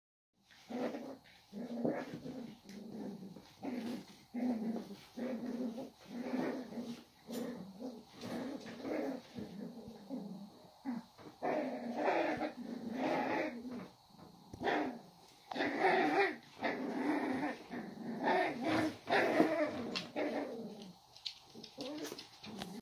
Puppies’ fight. Cavalier King Charles Spaniel puppies are fighting with each other.
Look how cute they are!